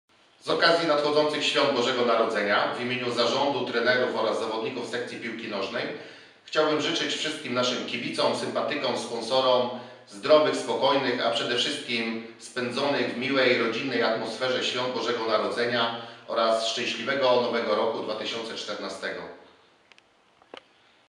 Życzenia od Nielby
Wiceprezes ds. piłki nożnej MKS Nielba Wągrowiec Andrzej Reis składa życzenia z okazji Świąt Bożego Narodzenia.